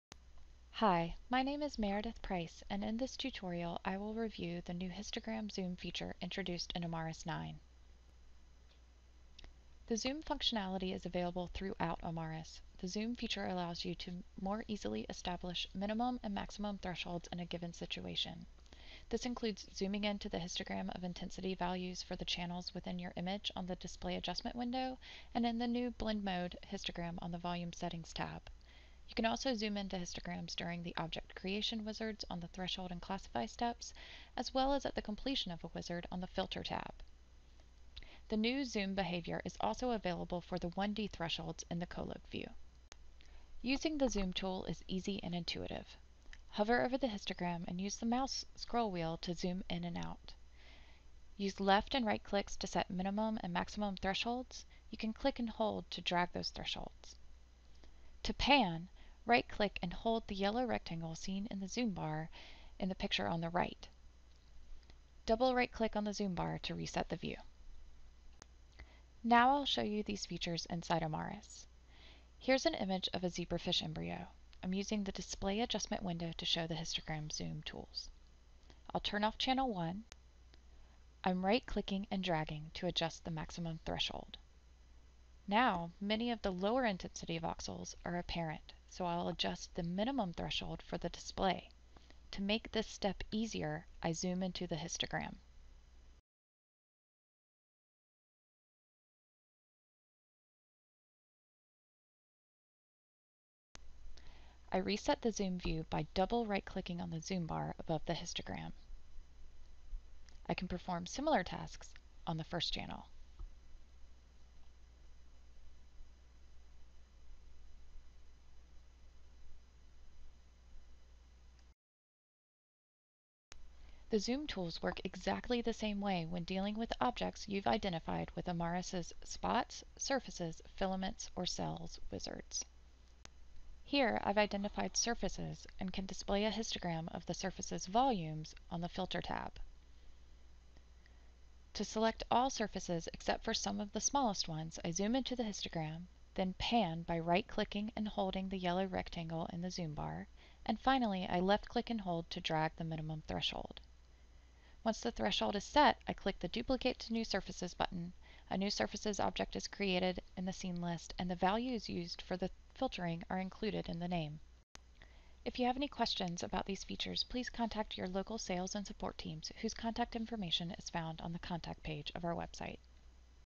How to improve the contract of your image?- tutorial
In this tutorial you will learn how improve your image contrast using precise histogram zoom function to adjust intensity levels of your microscopy images.